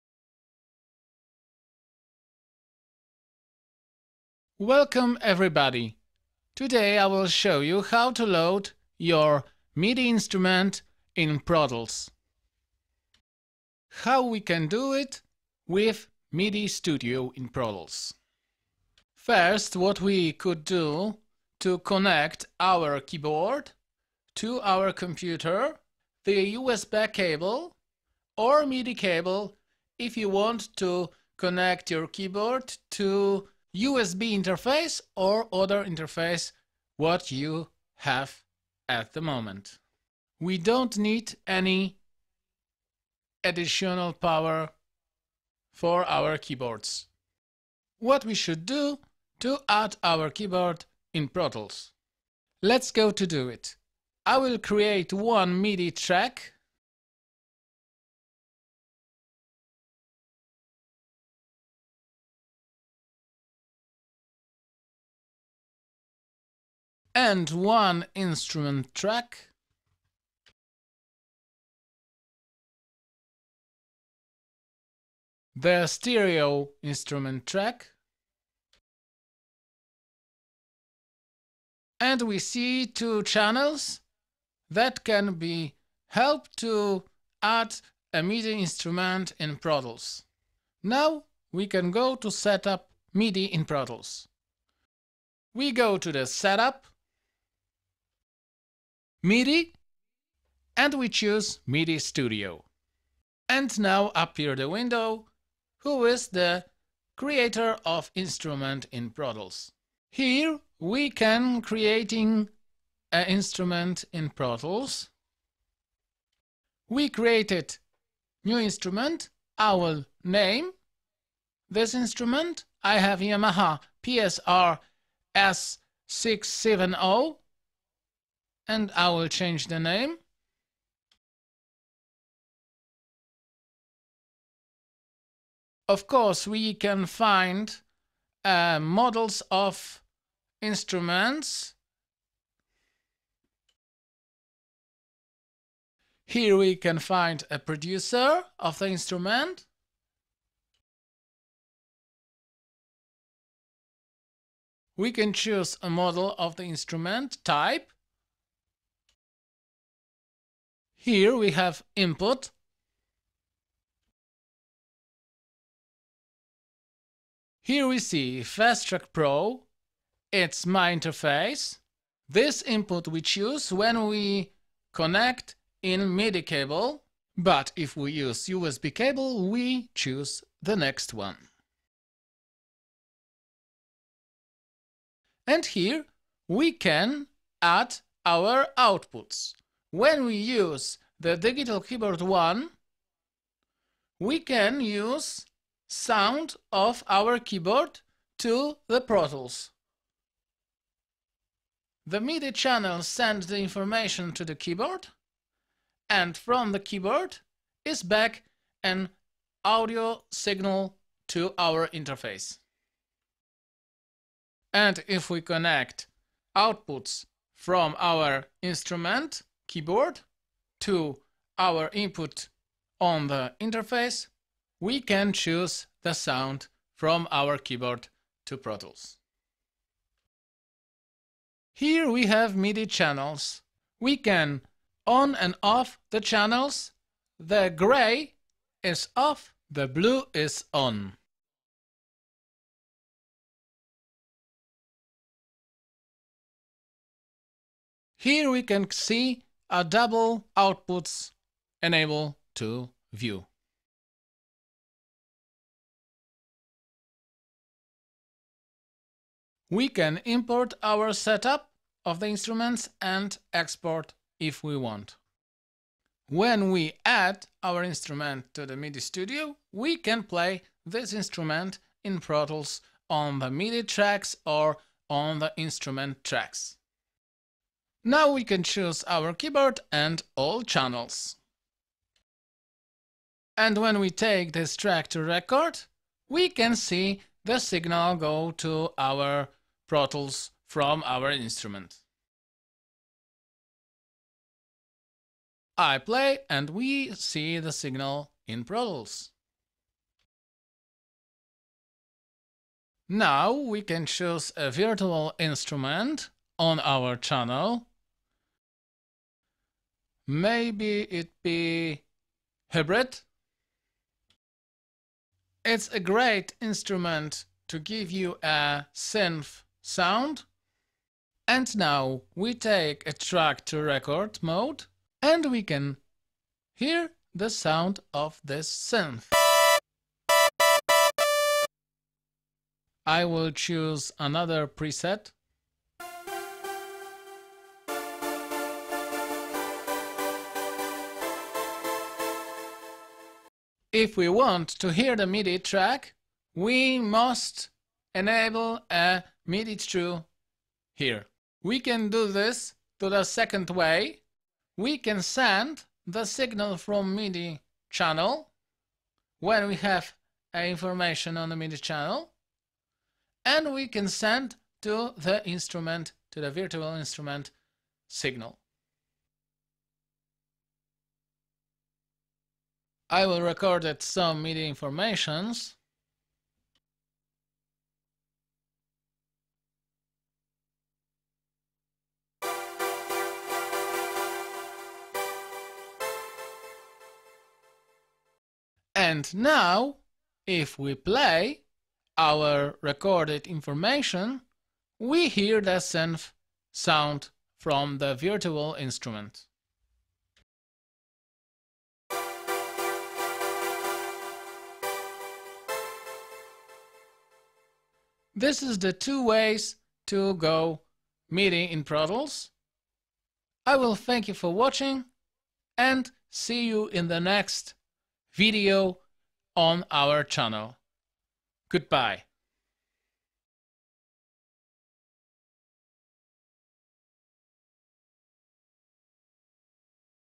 MIDI SETUP IN PRO TOOLS WINDOWS EN
This is a quick midi setup guide in pro tools. Look how it is easy.

Zapraszam do dawania donejtów, które w całości zostaną przeznaczone na rozwój kanału (w tym naprawa instrumentów,zakup aktualnie potrzebnego wyposażenia)